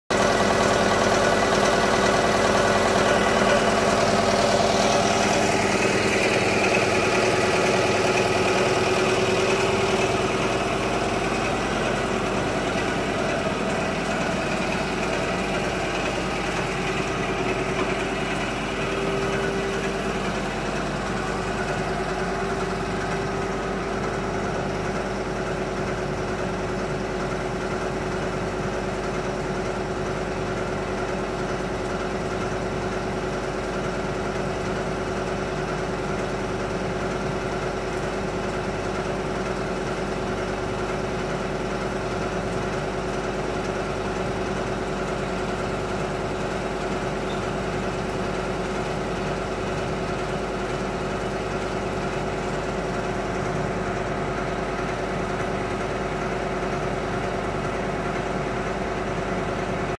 Desinfektion Gewächshaus mit Skumix Verschäumungsanlage durch Lohnunternehmen Guggisberg Zimmerwald
Dank Skumix Verschäumungsanlage perfekte Desinfektion von diversen Untergründen im Gewächshaus